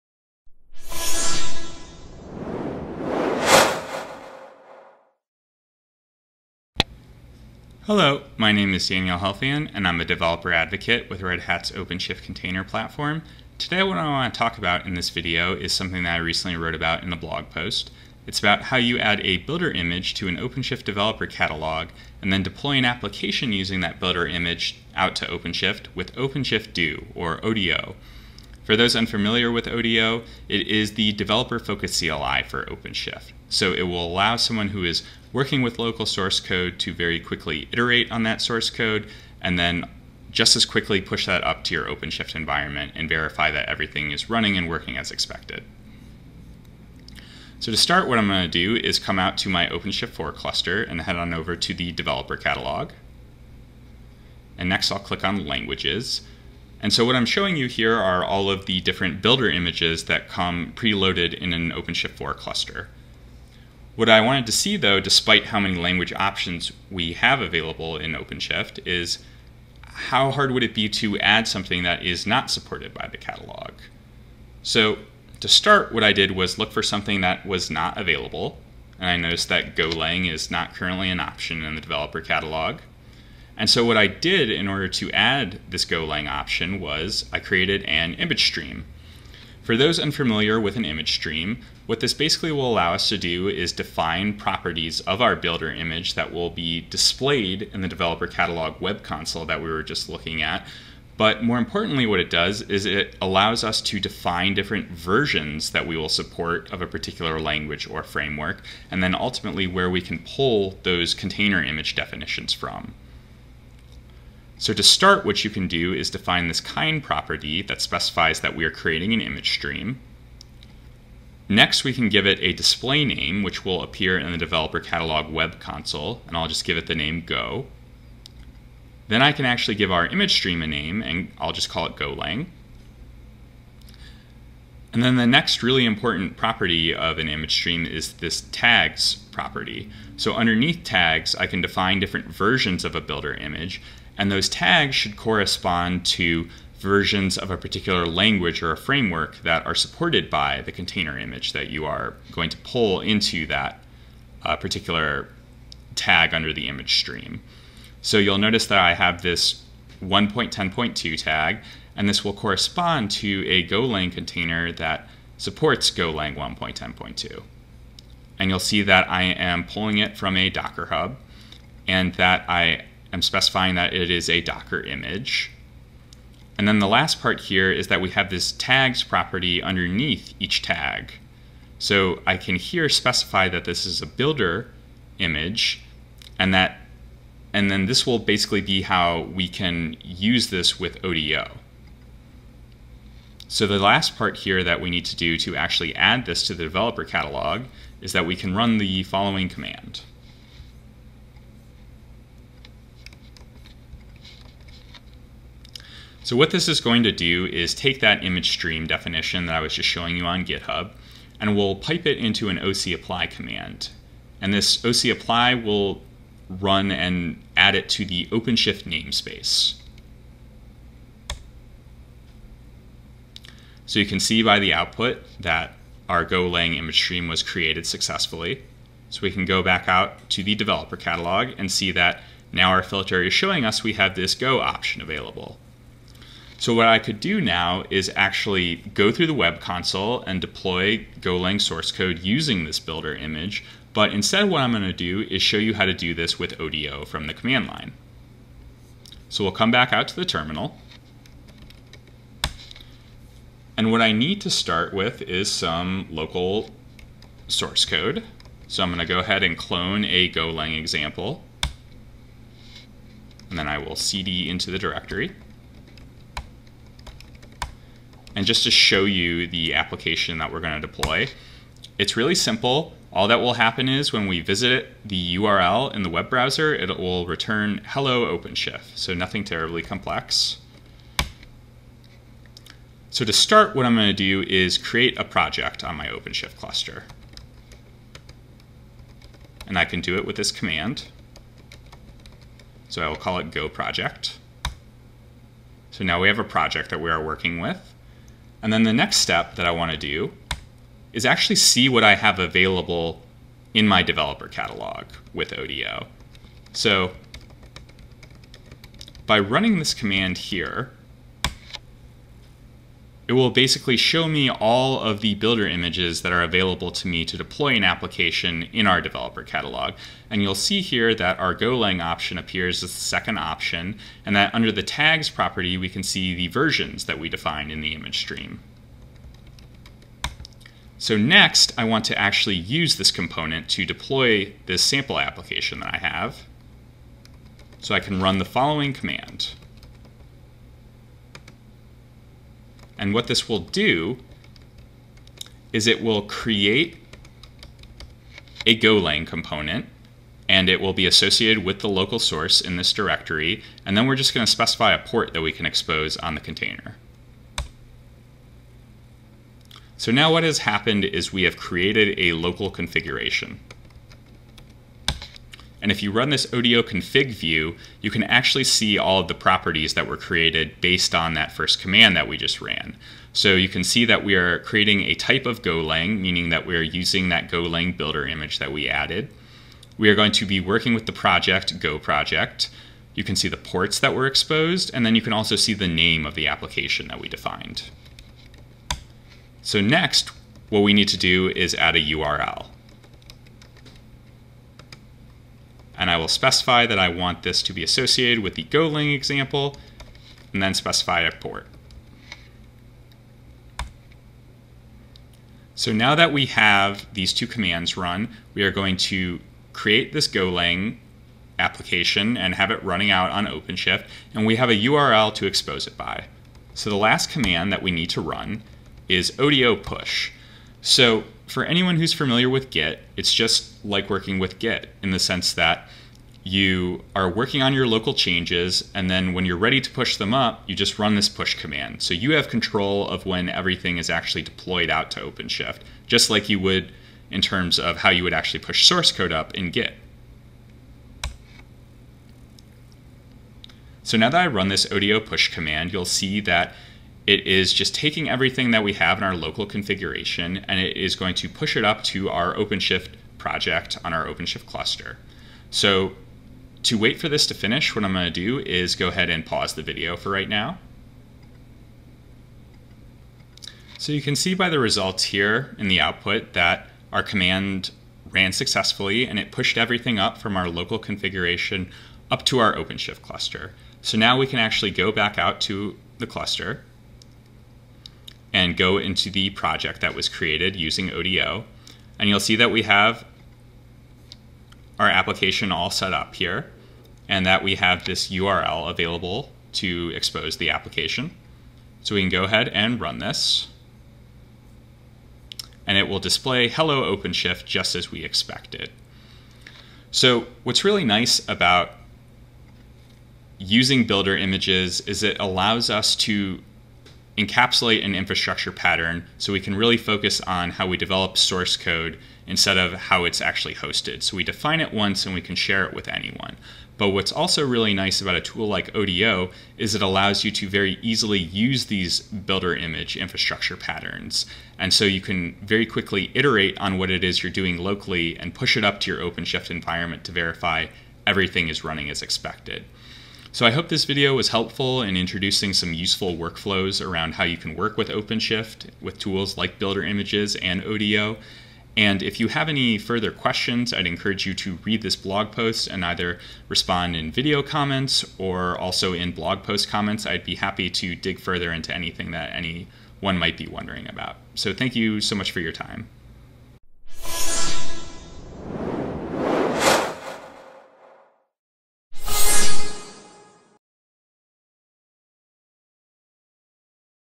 Using a custom builder image on Red Hat OpenShift
In this video, OpenShift Developer Advocate Daniel Helfand shows how to add a custom builder image that supports Go applications to an OpenShift 4 Developer Catalog. He then uses OpenShift Do (odo) to deploy a Go application using that builder image.